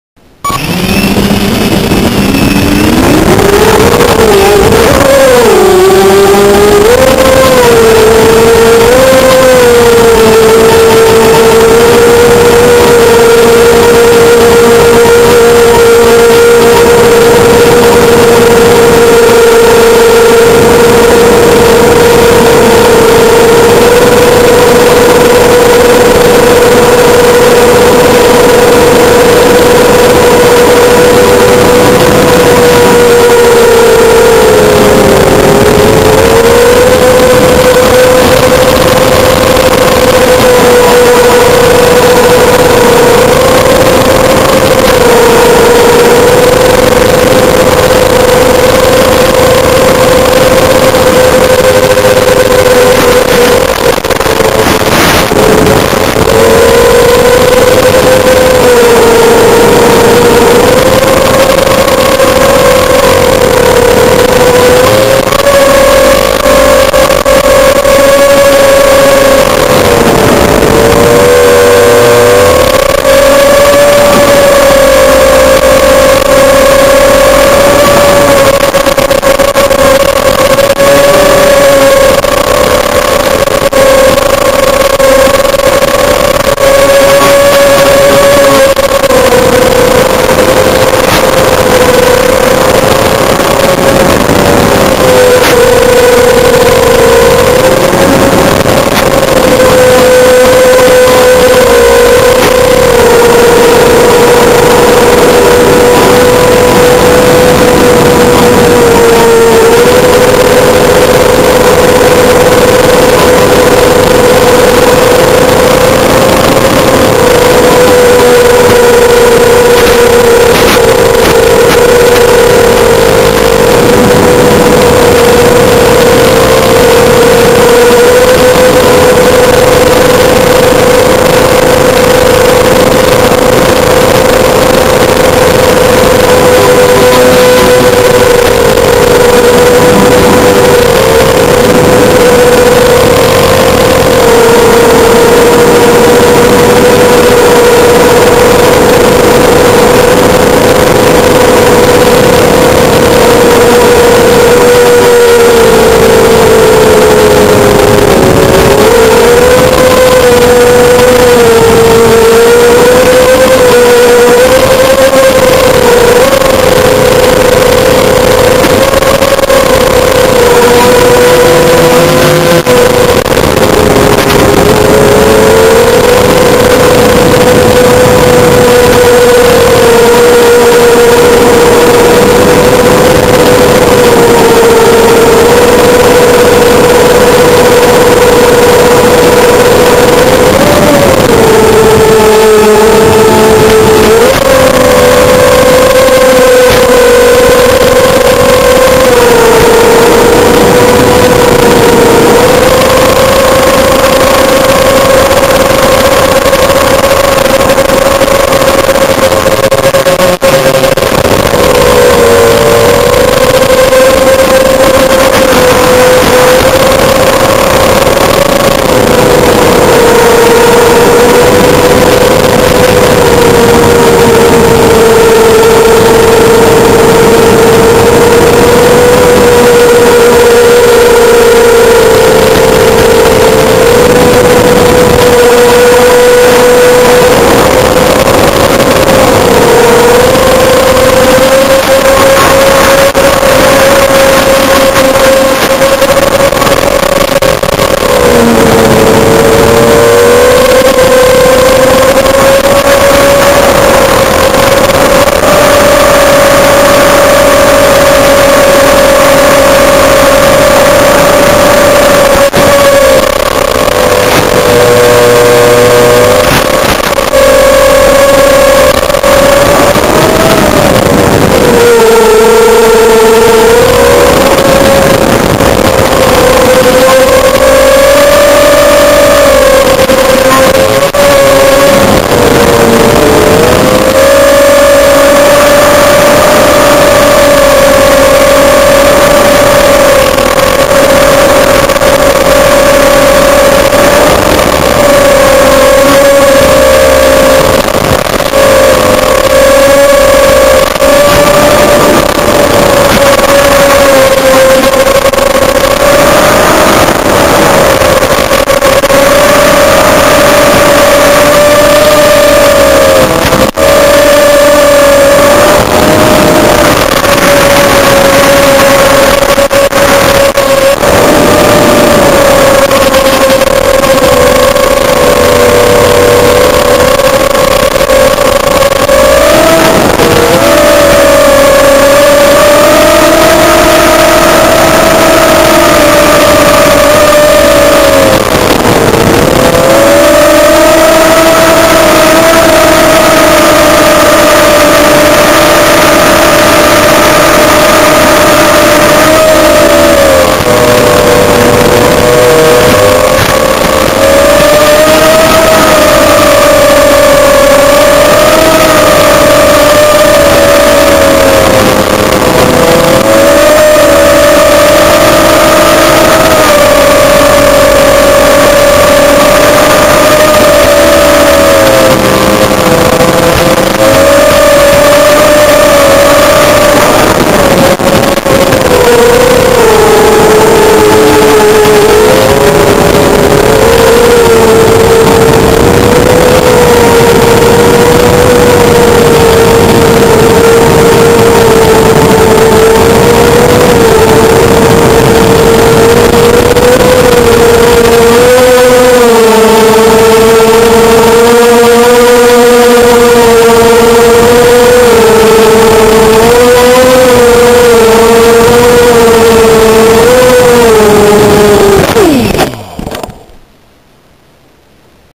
FPV250 Wring out
Had to see if I could get this $8.00 frame to go fast.